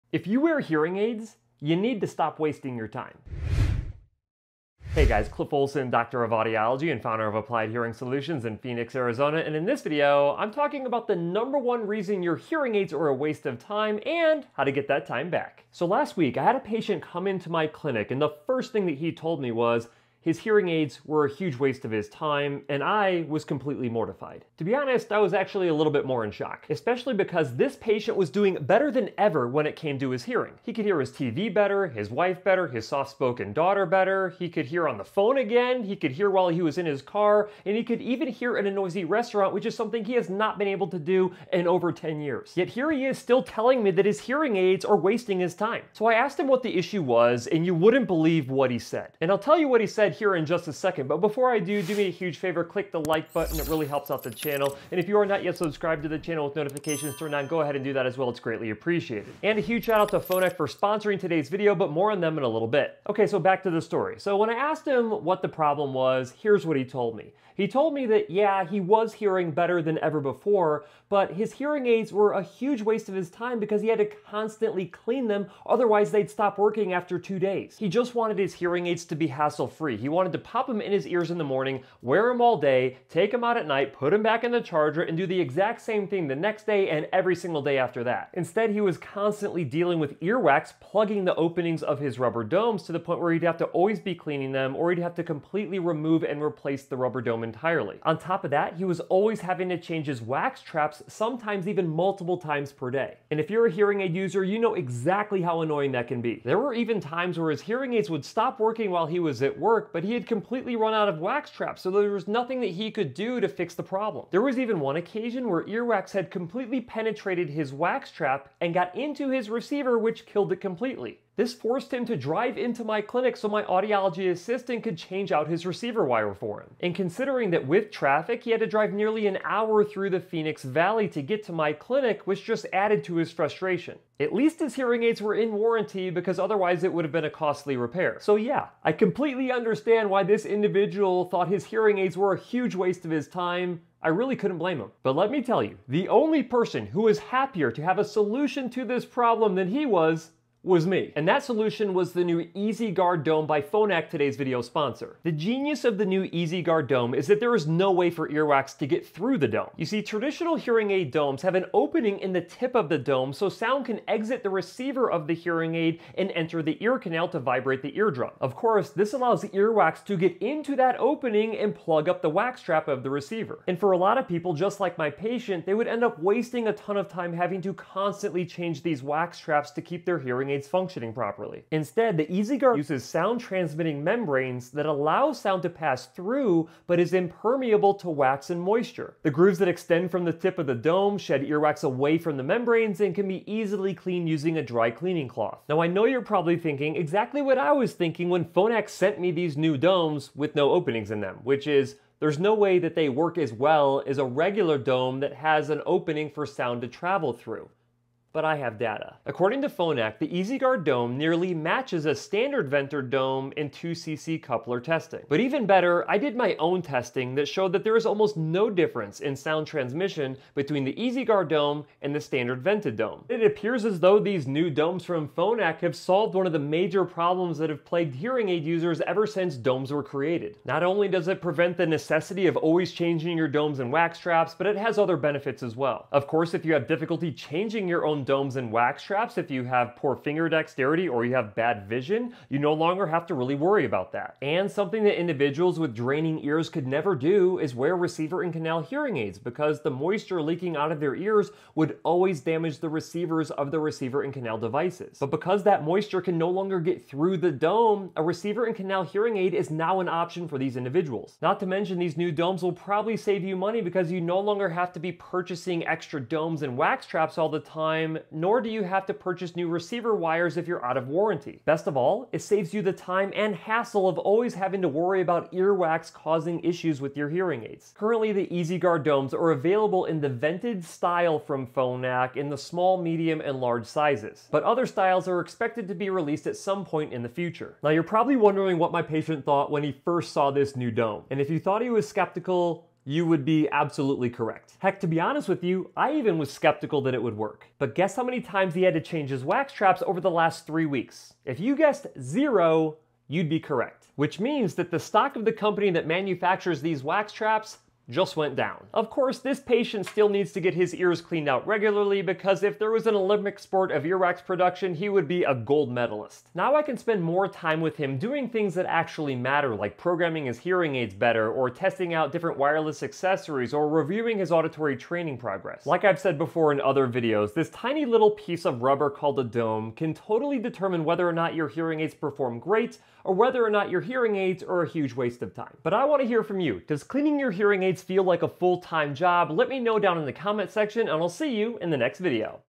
Hearing Aids are a WASTE OF TIME!
If you wear hearing aids, you need to stop wasting your time. In today’s video, Dr. Cliff Olson breaks down the #1 reason many hearing aid users feel like their devices are a “waste of time” — even when they’re hearing better than ever.

A real patient story reveals a surprisingly common problem: earwax clogging domes and wax traps, forcing constant cleaning and replacements. That frustration leads to downtime, missed conversations, and even broken receivers.

To solve this, Dr. Cliff reviews the new Phonak EasyGuard Dome — a design that blocks wax and moisture from entering the receiver while still allowing sound to transmit clearly. He explains how it works, what the data says, and why it could be a game-changer for:

People constantly changing wax traps
Anyone with poor vision or finger dexterity
Hearing aid users with draining ears
Those who want truly hassle-free devices

Have you ever felt like cleaning your hearing aids is a full-time job? Drop your experience in the comments!